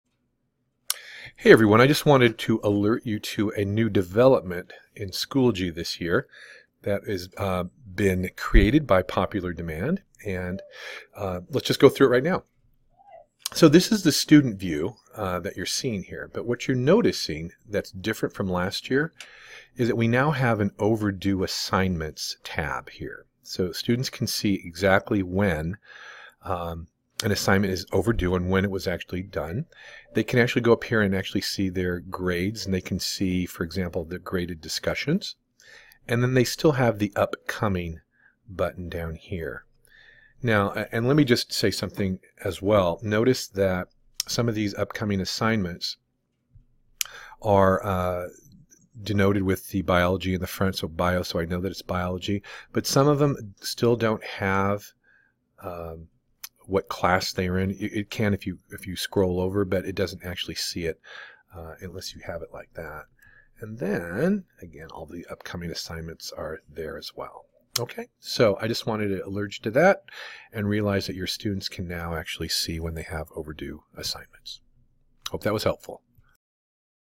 Schoology: Overdue Assignments
New feature in Schoology (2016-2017) Overdue assignments alert